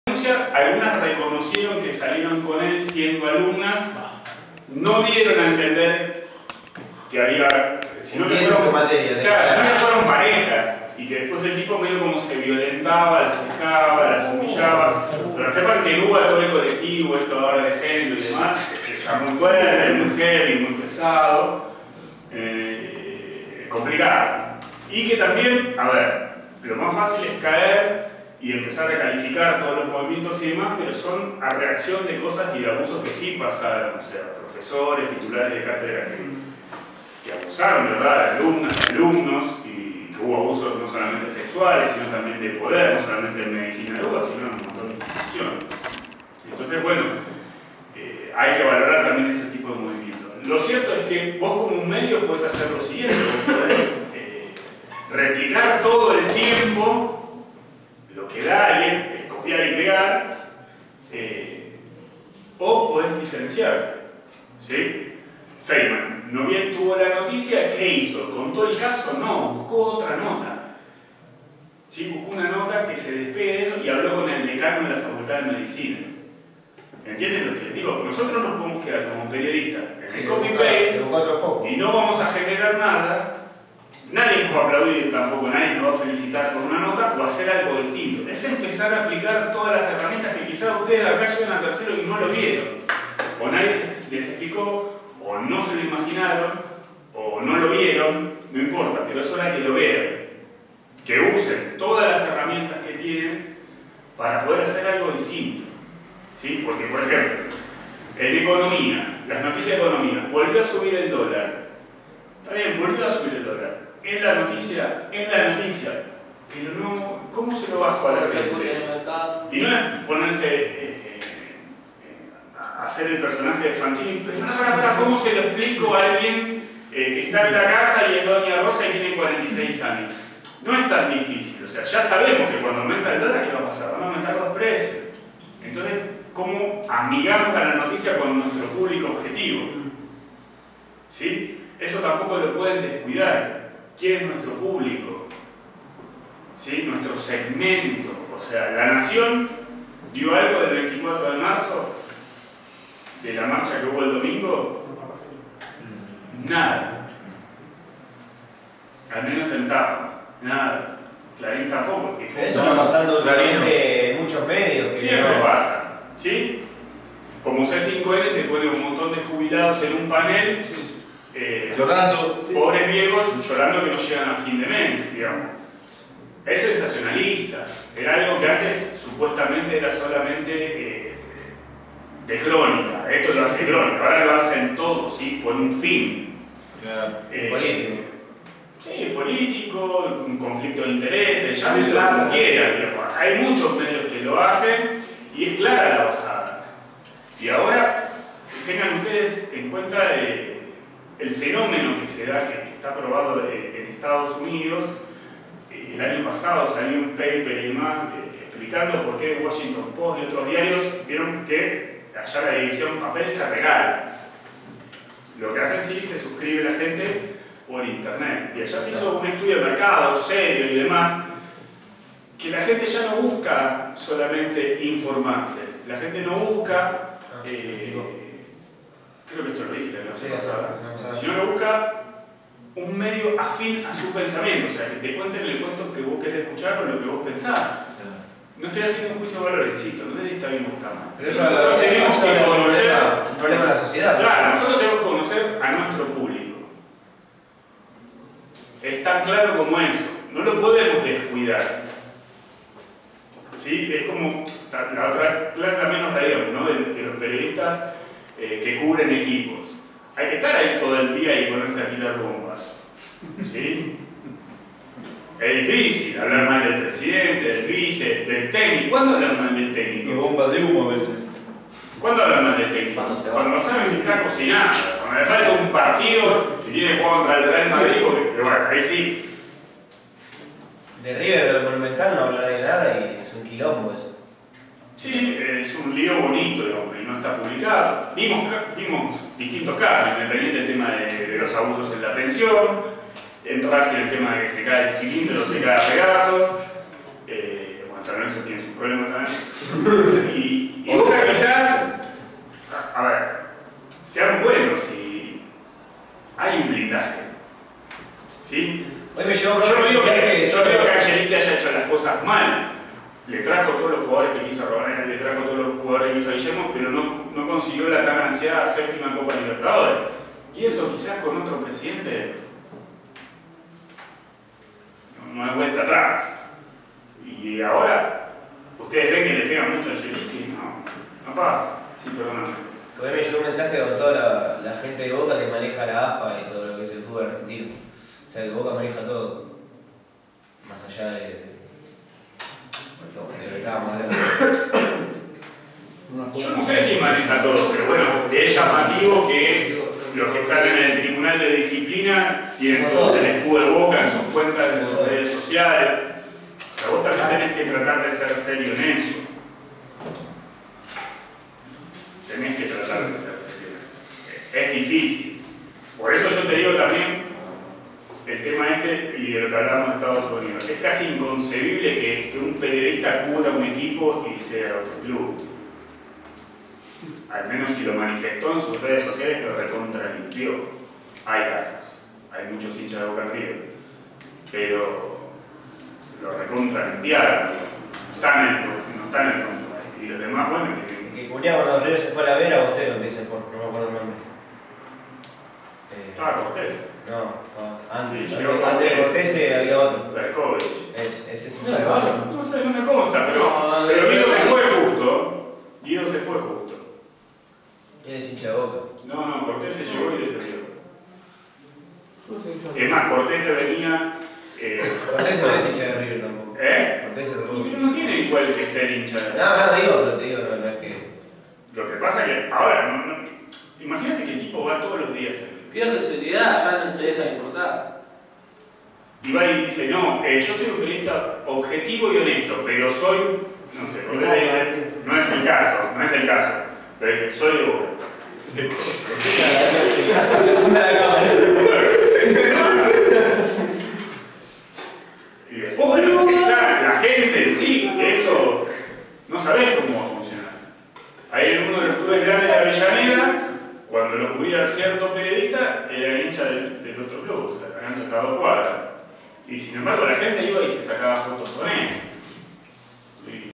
Martín Dovgaluk clase Análisis de la Actualidad en el ISPeD. Parte I.
Clase a cargo del periodista e integrante del Instituto Superior ISPeD Martín Dovgaluk. El docente, pone manos a la obra a los alumnos de 3ro división B del turno noche 2019.
Breve y divertido resumen.